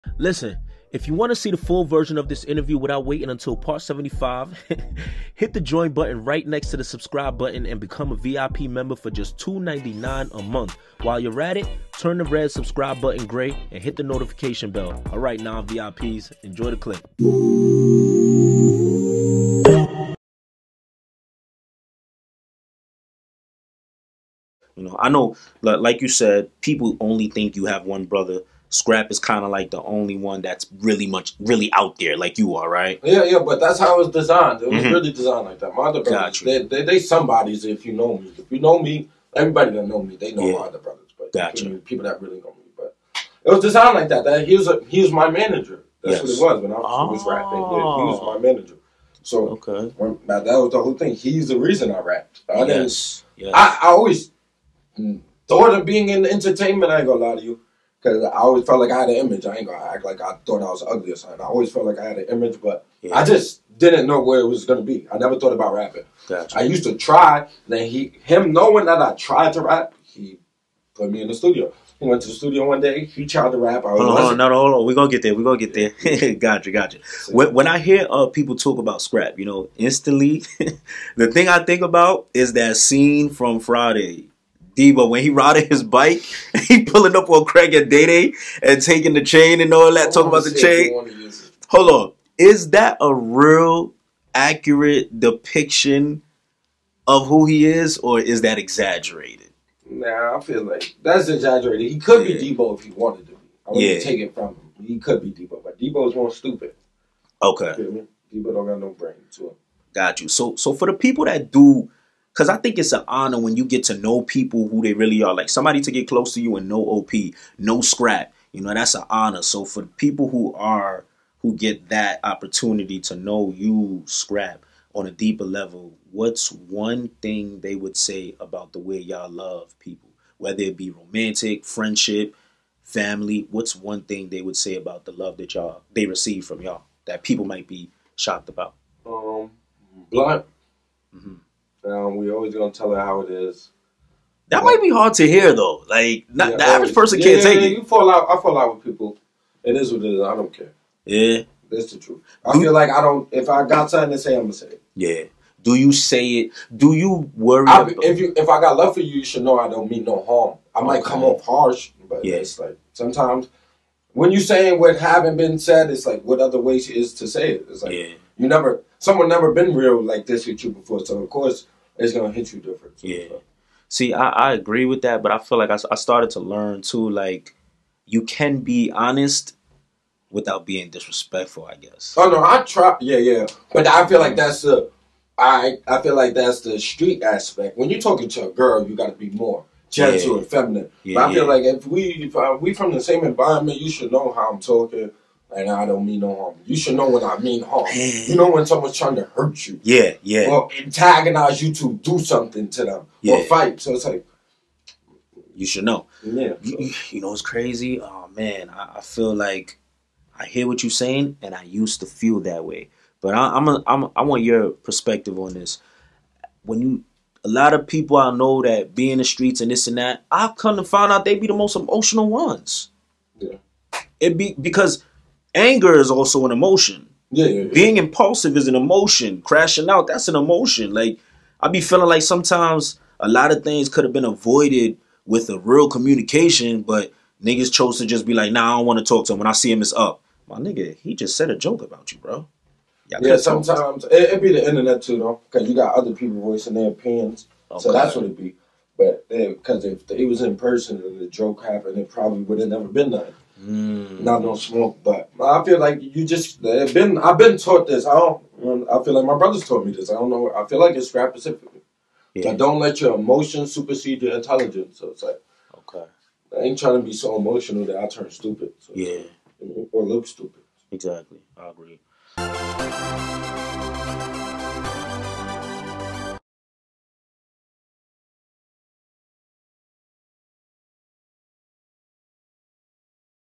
OnPointLikeOP On His Relationship With His Brother Skrap 1090 : "He's The Reason I Rap" (Pt.8)
In this part of the interview, OnPointLikeOP speaks on Skrap1090.

PPhilms stays tapped in with your favorite artists and creatives.